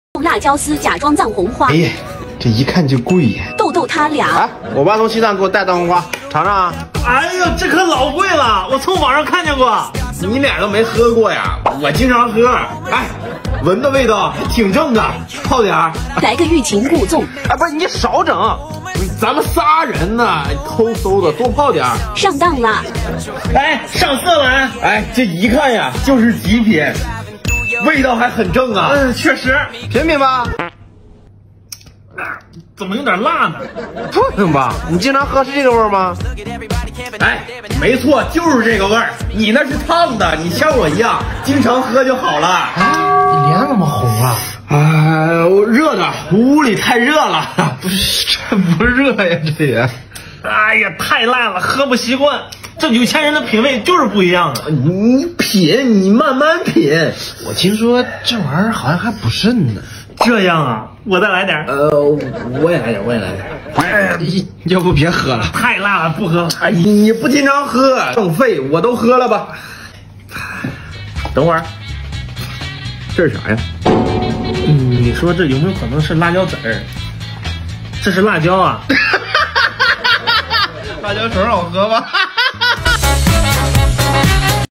峻宁峻宁：把辣椒丝当藏红花喝！卧龙凤雏凑齐了你俩
我们的视频是原创作品，绝对没有任何不良引导或危险行为。 哈喽大家好，我是峻宁~